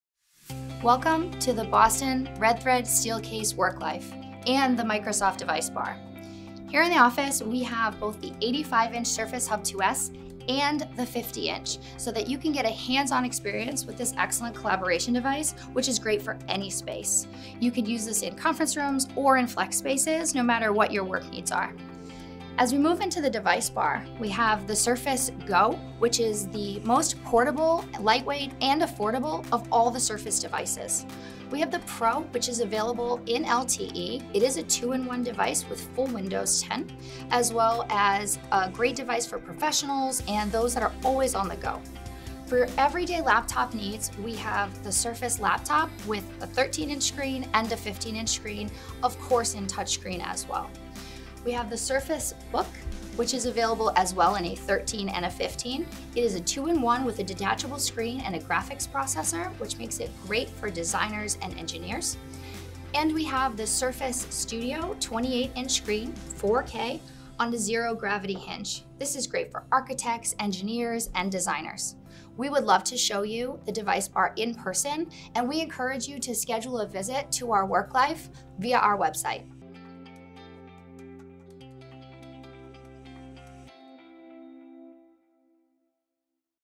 Boston WorkLife Microsoft Device Bar
No matter your work needs, Microsoft has you covered.

Whether you’re looking for a collaborative tool to power your large conference room like the 85’’ Surface Hub 2S, or a portable and lightweight product to support everyday laptop needs like the 10.5’’ Surface Go, our Microsoft Device Bar has it all.

Visit our Boston WorkLife today for a hands-on experience of all the versatile Surface devices that can enhance employee productivity through speed, graphics, and performance combined.